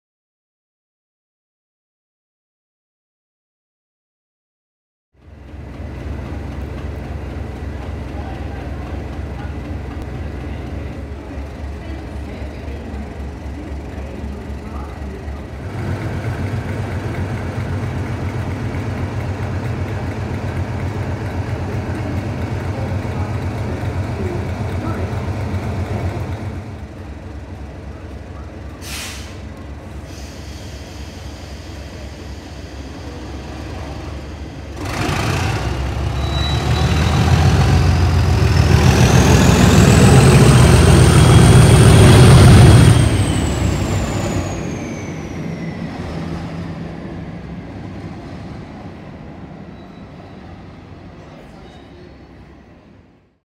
DRS 37419 guns it away from Ipswich working 0V91
DRS 37419 guns it away from Ipswich working 0V91 from Norwich to Ilford.